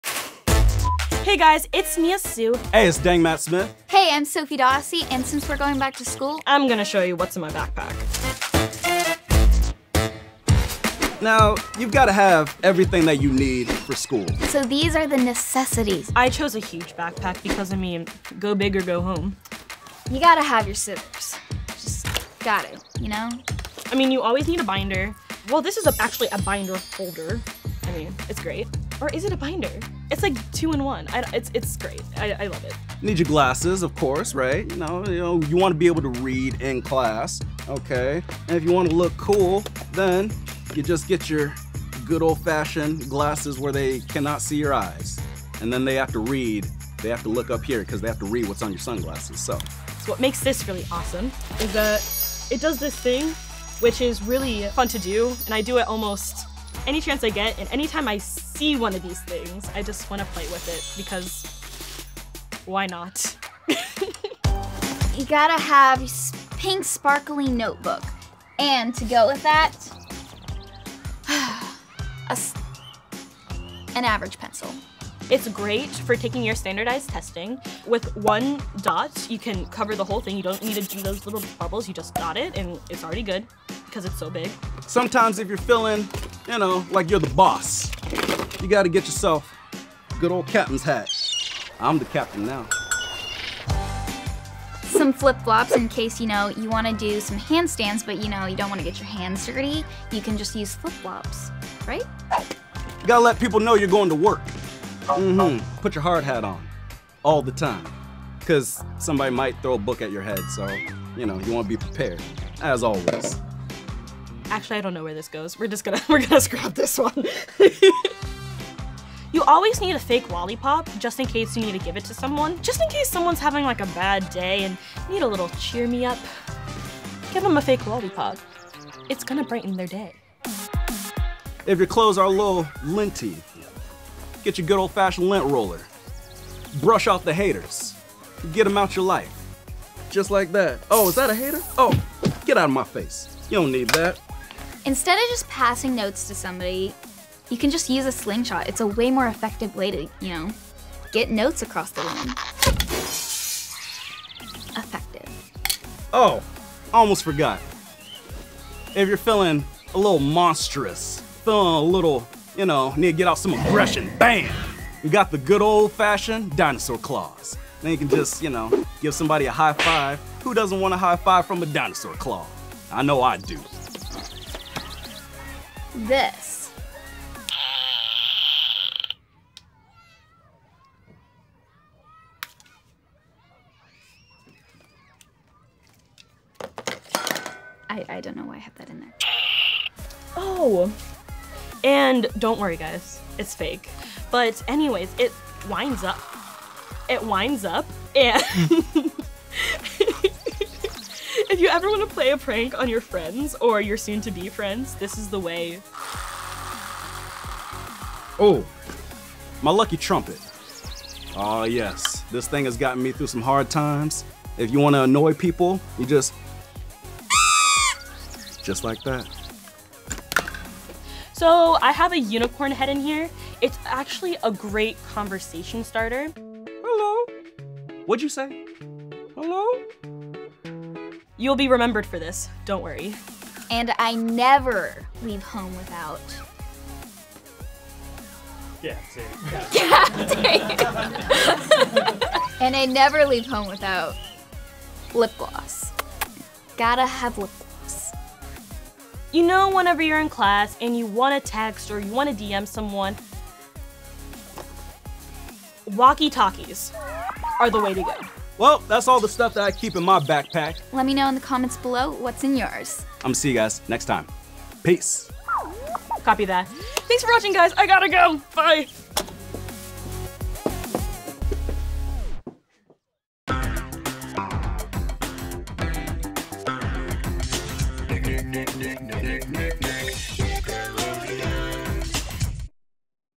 BackToSchool 🎒 What’s in My Backpack! ft. DangMattSmith, Sofie Dossi & Nia Sioux | Nick
It’s back to school time!! Curious what your favorite influencers have in their backpack? 🎒 Watch as Dang Matt Smith, Sofie Dossi and Nia Sioux share their backpack essentials they need for a day of school. What do you have in YOUR backpack? Catch more of your favorite shows on Nick!